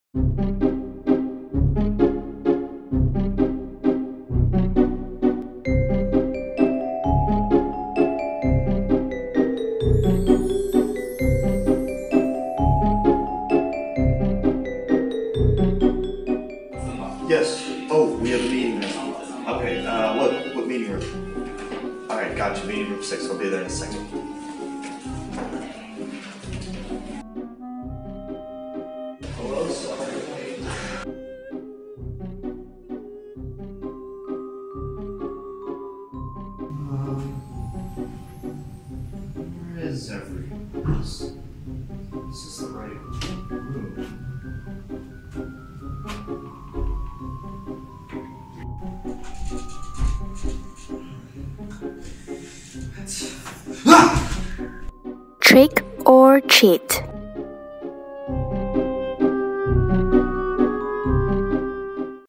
Happy Halloween
Happy Halloween from Sanbot and all of us at QIHAN Technology! Sanbot is playing some classic Halloween pranks on some of our QIHAN staff members, enjoy!